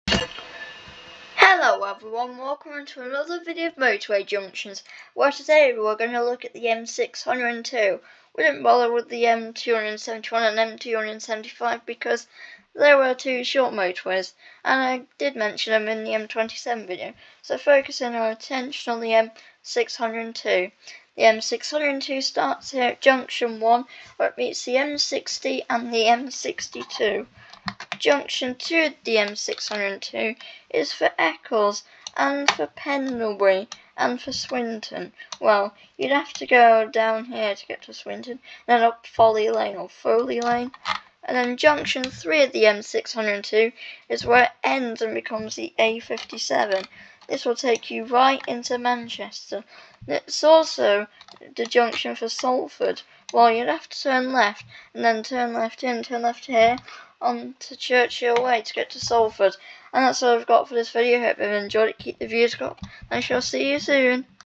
Motorway Junctions - M602
In this video, I show you all the motorway junctions on the M602